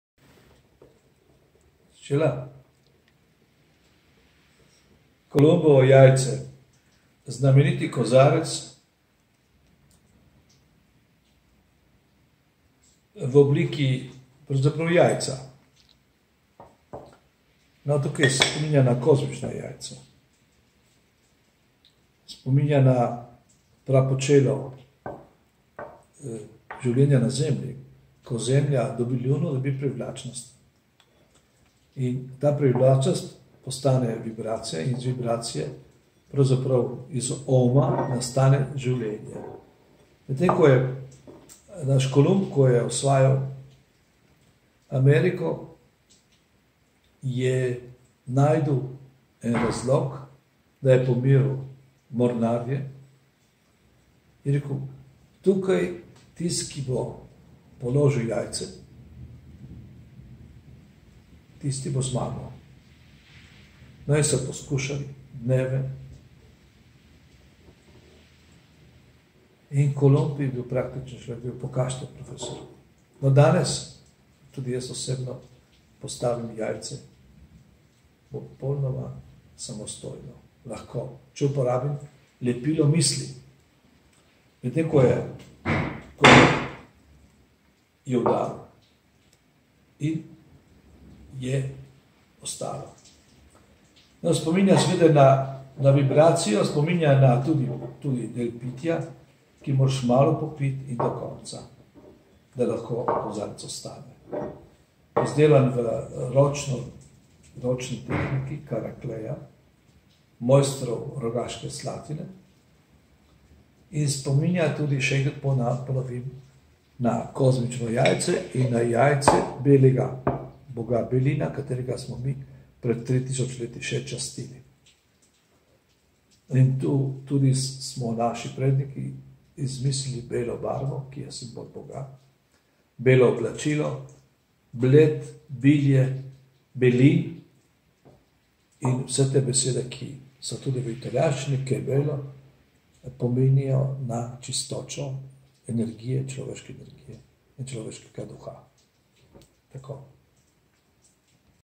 OSKAR KOGOJ, KOLUMBOV KOZAREC, simbol človeškega življenja / COLUMBUS’ GLASS, symbol of human life
KOLUMBOV KOZAREC ZA VISKI, simbol človeškega življenja ...

Izhaja iz oblike jajca, nanaša pa se na odkritje Amerike. Znana pripovedka razlaga, kako je Kolumb na razburkanem morju tolažil svoje mornarje. Obljubljal jim je, da tistega, ki postavi jajce pokonci, nagradi. Te filozofsko obarvane naloge ni zmogel nihče, razen njega. Prijel je jajce in z njim udaril po mizi – ostalo je pokonci. Na to jajce spominja Kolumbov kozarec, ki se nenehno vrti, premika in vibrira kot človeško življenje, Zemlja.
Kozarec s tekočino je potrebno držati v roki, sicer se tekočina zlije. Hudomušen kozarec, ki ima svoj čar!
IZDELEK JE ROČNO DELO!

COLUMBUS’ WHISKEY GLASS, symbol of human life ...

Its shape is derived from the oval, egg-like shape that is connected symbolically to the discovery of America. According to a well-known tale, Columbus tried to solace and cheer up his sailors during a storm by distracting them from thinking about the rough sea. He promised that whoever succeeded in putting the egg upright would be awarded. No one but him was able to perform this philosophically tinged task. He took the egg and tapped it on the table to flatten its bottom – it remained upright. The Columbus glass reminds us of this egg, whirling perpetually, moving and vibrating incessantly just like human life, the Earth.
The glass with the liquid should be held in the hand, otherwise the liquid will pour. A playful glass that has its charm!
THE PRODUCT IS HANDMADE!